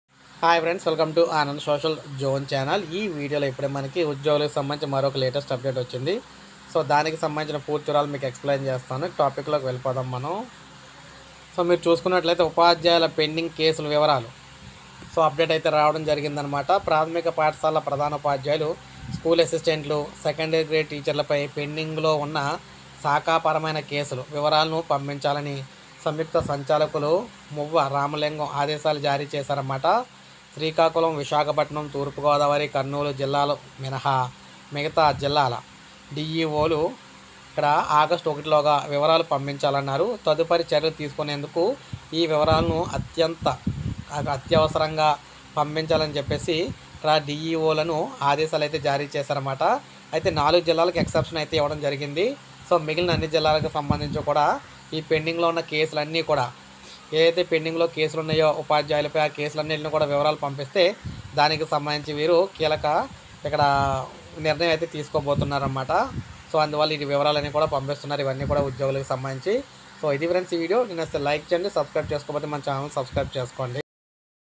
రాష్ట్రంలో ఈ ఉద్యోగుల పెండింగ్ కేసులు పై కీలక నిర్ణయం ap employees latest update on pending cases.
hello everyone today update is AP government latest update on employees .@Anandsocialzone